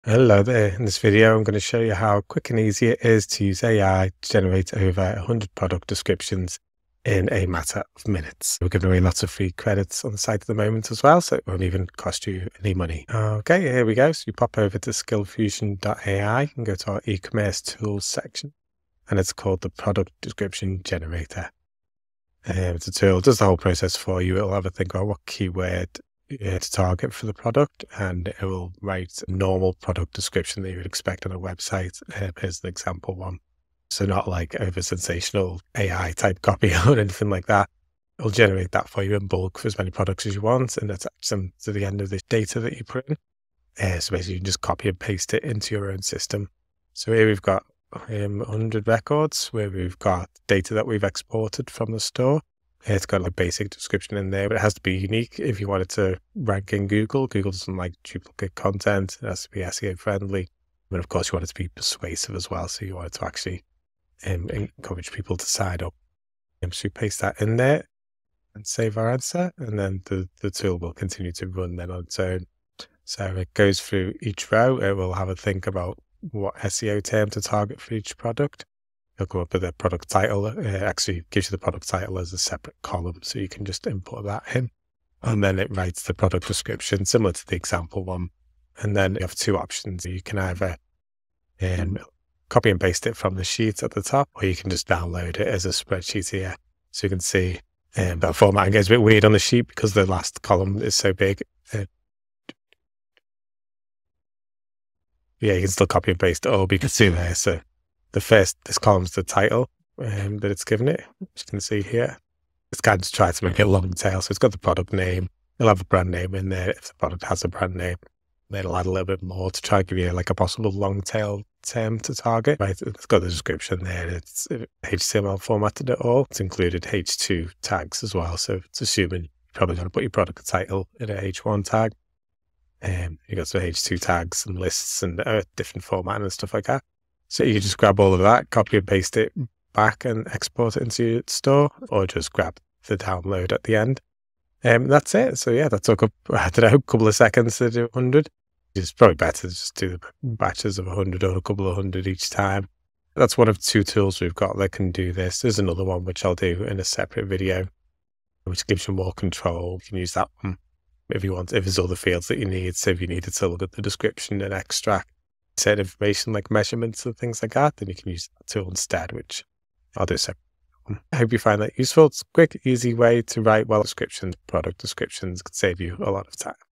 How To Generate 100 Product Descriptions in 1 minute With AI
Case study on how to generate 100 SEO optimised, sales optimised and html wrapped product descriptions. Using Skillfusion AI